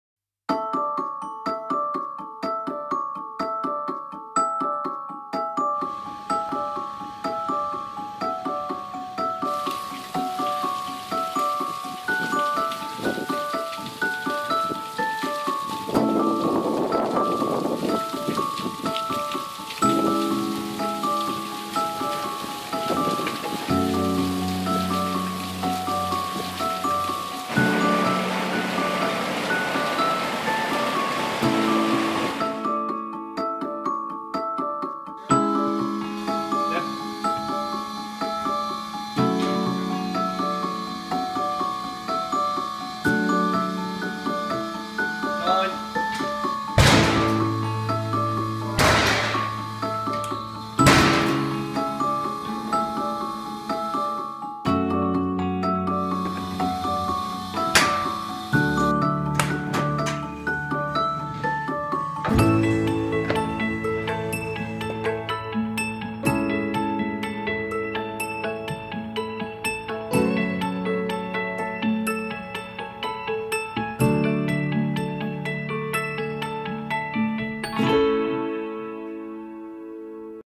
Window Glass Independent Testing Of Products For Security And Weather Performance
Video clips demonstrating the independent UKAS and CWCT testing of window glass products including windows, doors and curtain walling using aluminium framing systems. Using graphics and framing to make the most of amateur footage, includes the use of informative and educational text.